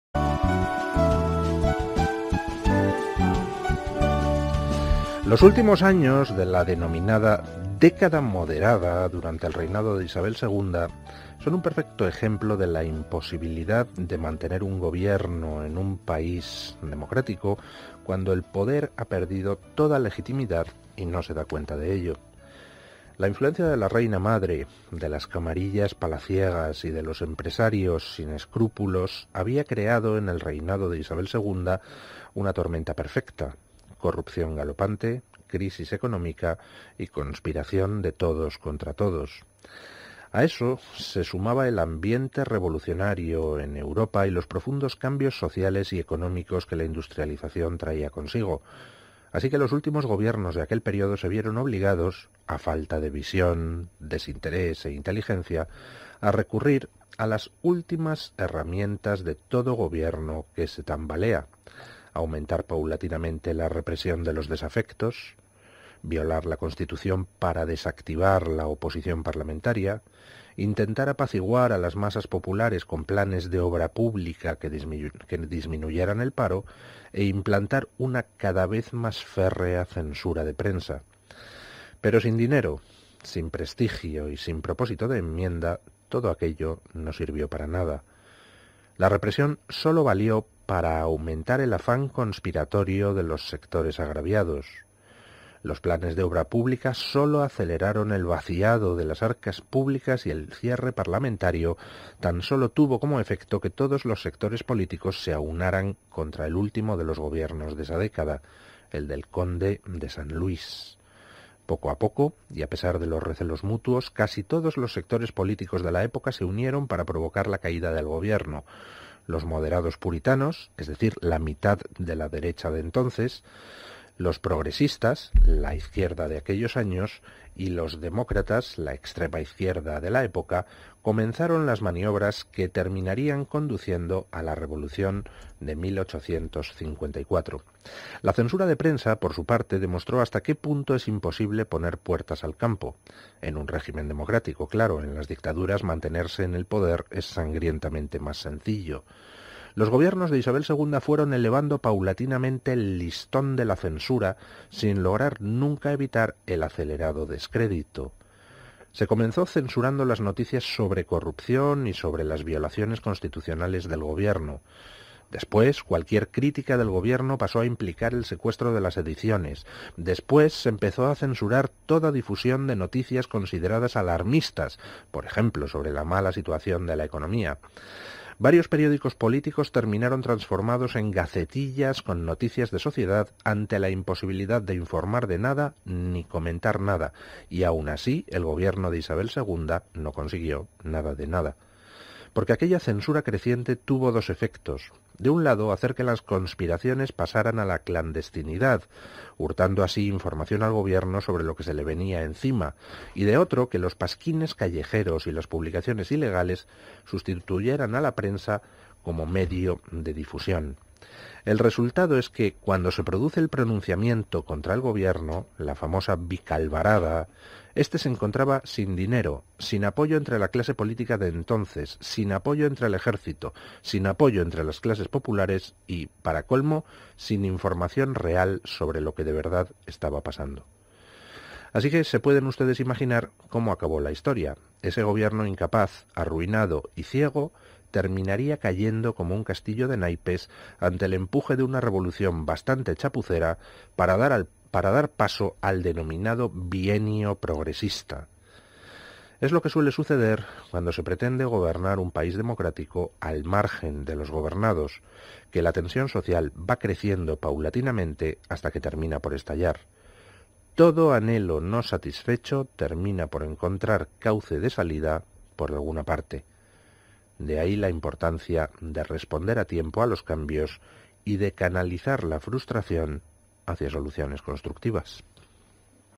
Editorial de Luis del Pino: "Los prolegómenos de la Vicalvarada"
Es imposible mantener un Gobierno en un país democrático cuando el poder ha perdido toda legitimidad y no es consciente de ello...